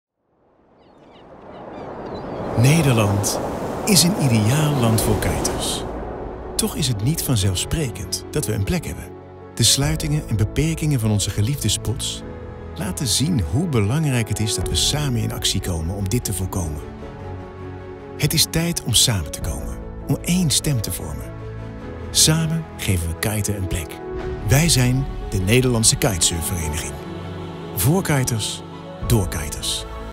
NKV [reel] brandvideo snippit
Het is tijd om samen te komen.
Om één stem te vormen.
Samen verantwoordelijkheid te nemen voor het behoud, de verbetering en het openen van nieuwe kitesurfspots.

Om dit voor elkaar te krijgen
en zorg te dragen voor onze sport,
Is het nodig om samen een vuist te maken en kiten díe plek te geven.

Hoe meer stemmen we hebben, hoe meer slagkracht.

Zonder jou gaat dat het niet lukken.

Bekijk de hele brandvideo en ontdek waar wij voor staan en waarom we jou nodig hebben op ons YouTube kanaal in de playlist NKV VIDEO.